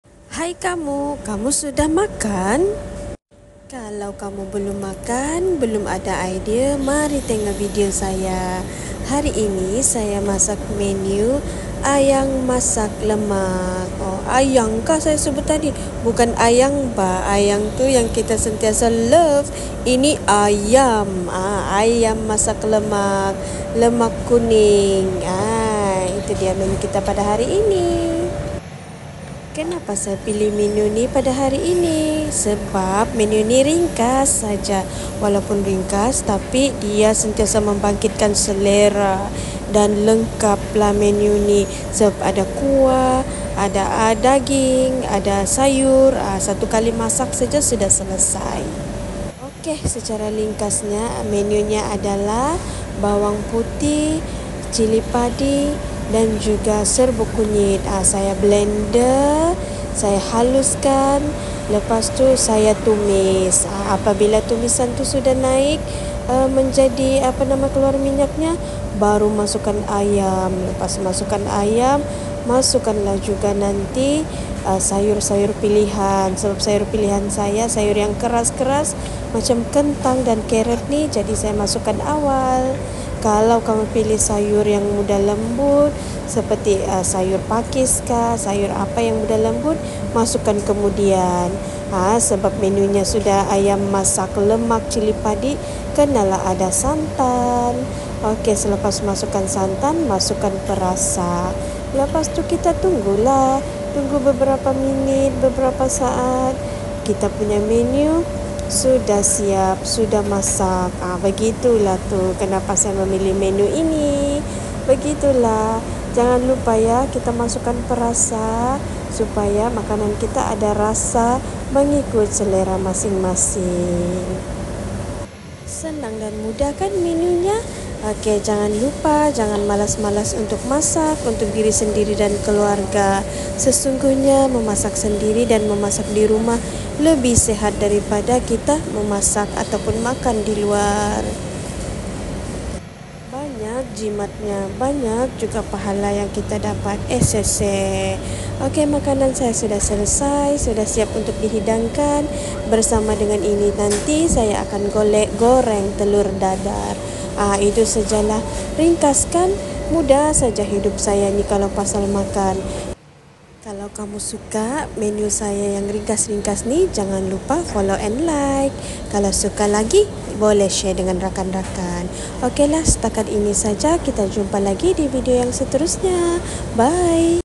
Ayam masak lemak cili padi
Menu ringkas untuk dijamu kepada seisi keluarga. Setiap hari memasak, sebagai suri rumahtangga menu pilihanku semestinya yang ringkas tetapi enak. Mari saksikan bersama.